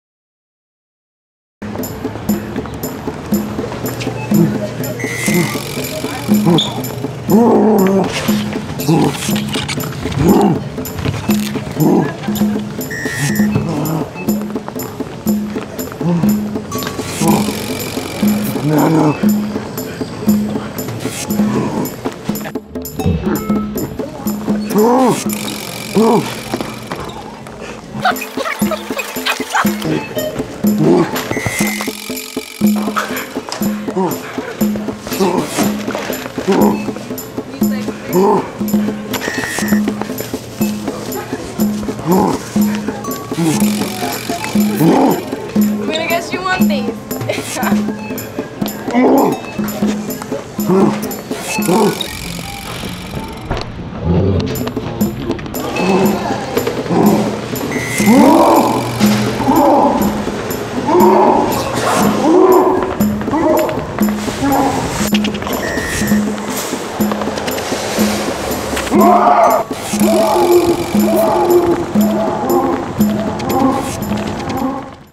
Gorilla Grocery Shopping
YNIL in Salina, Kansas killing time before the show when Doug gets a hold of a gorilla suit and does some grocery shopping! check out